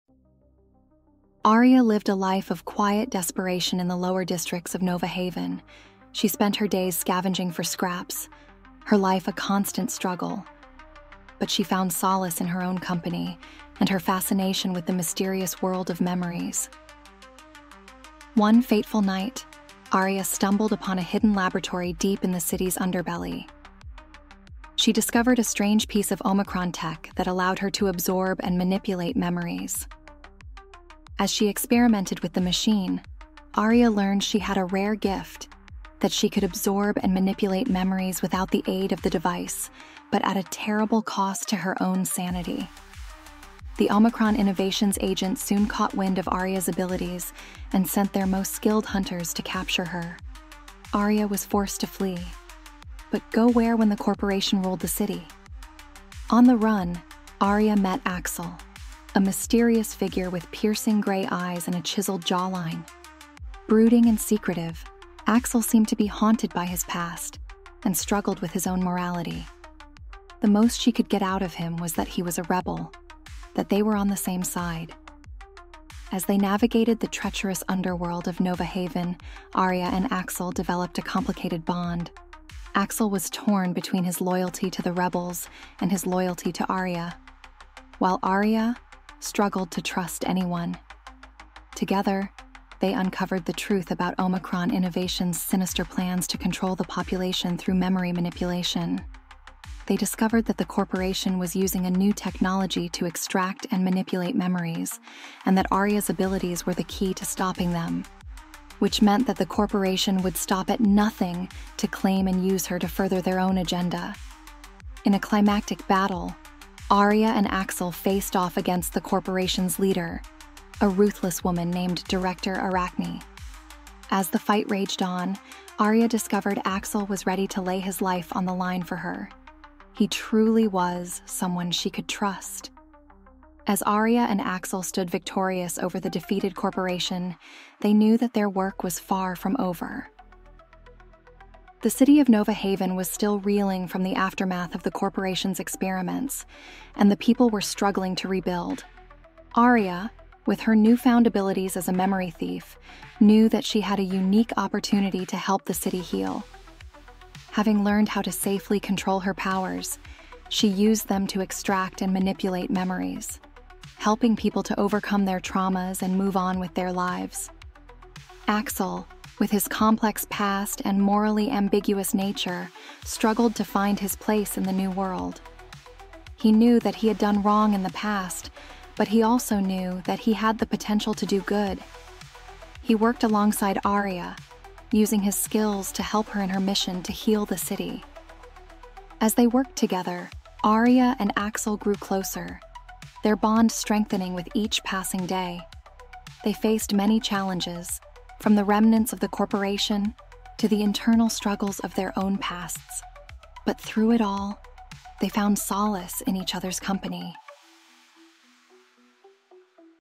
The Memory Thief - Cyberpunk Mystery Adventure Romance
📚👀 Are you ready for a story that will keep you on the edge of your seat?

🎥 "The Memory Thief" is a gripping tale of a young woman who can steal and manipulate memories.

🔮 With her unique gift, she must navigate a dark and treacherous city to uncover the truth about her past and the secrets that lie within. 💥